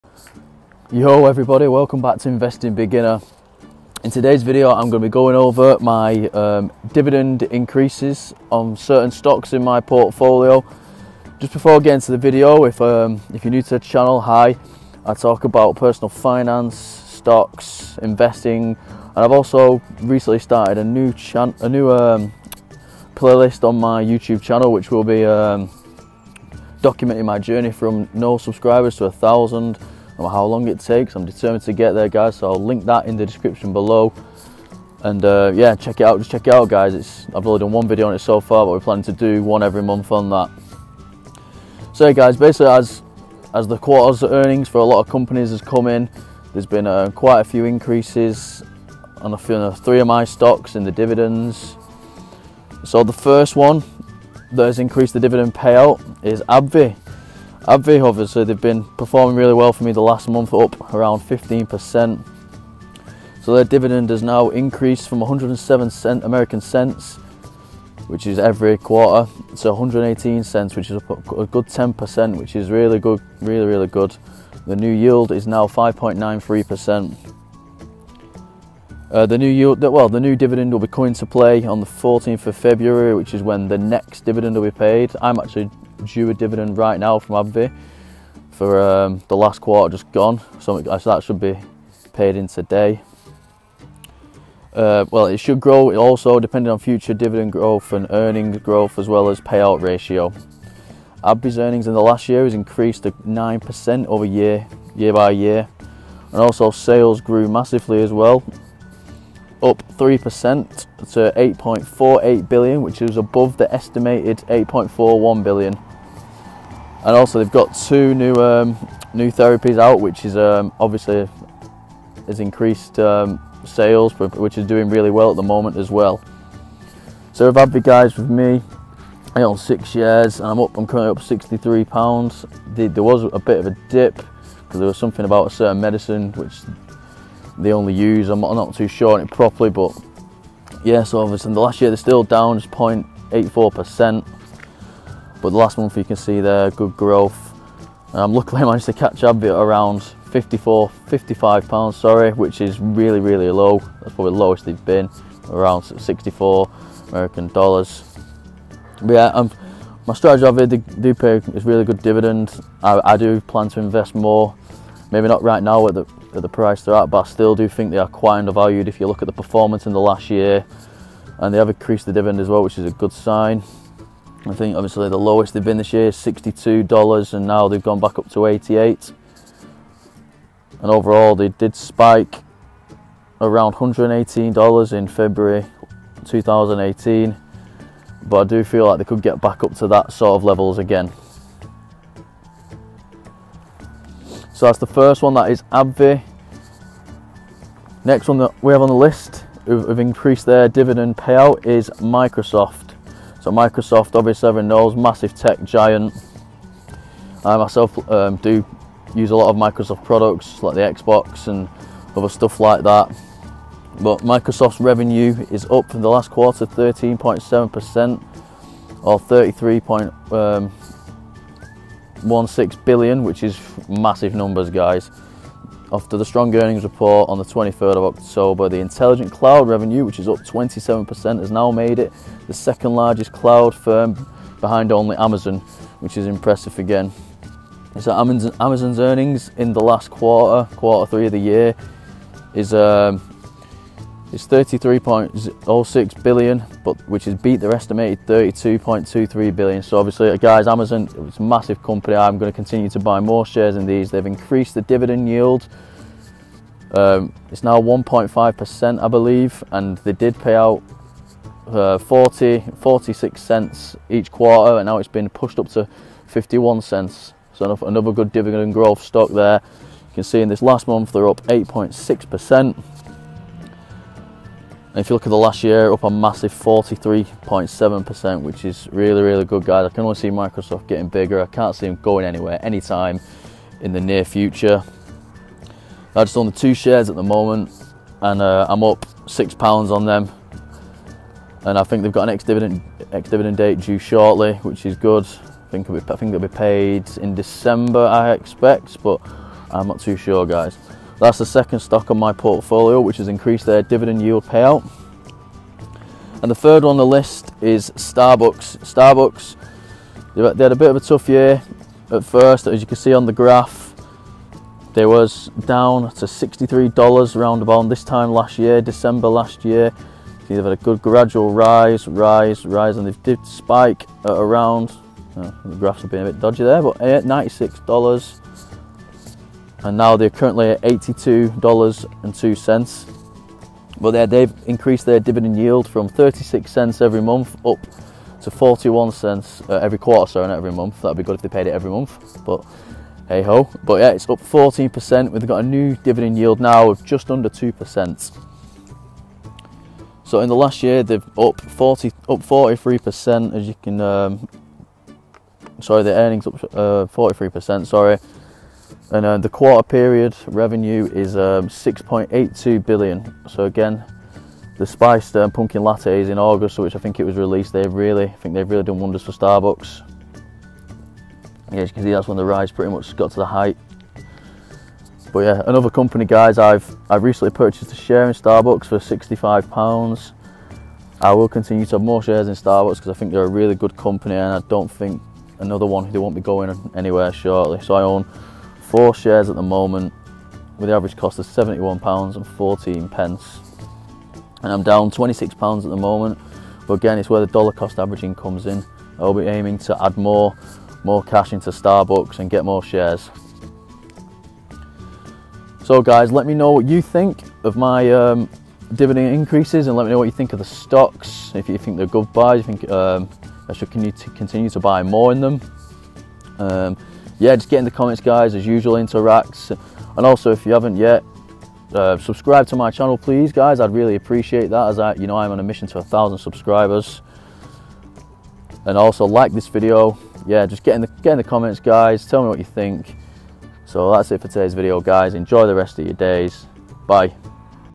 My Stock Dividend Increases on the freetrade UK investment app | Dividend Investing UK
Hi guys in today’s video lll be going over Dividend stocks in my portfolio that that have increased their dividend payouts the 3 stocks that have increased their dividend payment are The Microsoft stock (msft stock) the Starbucks stock (SBUX stock)  and finally the Abbvie stock (abbv stock)

These are stocks I plan for gold for the long term so to see that they are Inceasing their dividend is awesome